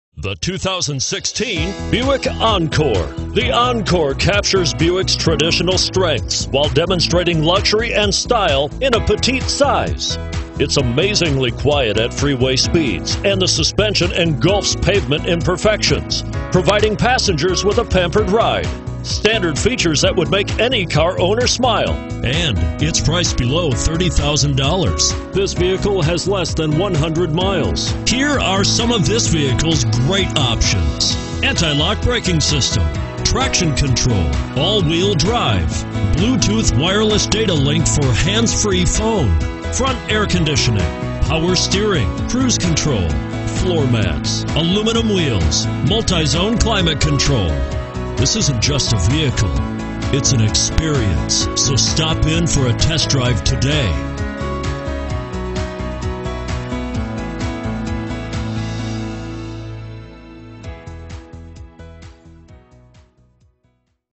2016 Buick Encore Minneapolis, St Cloud, Elk River, Monticello, MN B16-77
2016 Buick Encore Sport Touring

Don't miss this 2016 Buick Encore. It's equipped with Automatic transmission and features a GRAPHITE GRAY METALLIC exterior. With 9 miles, You'll want to take this suv home. Make a great choice today.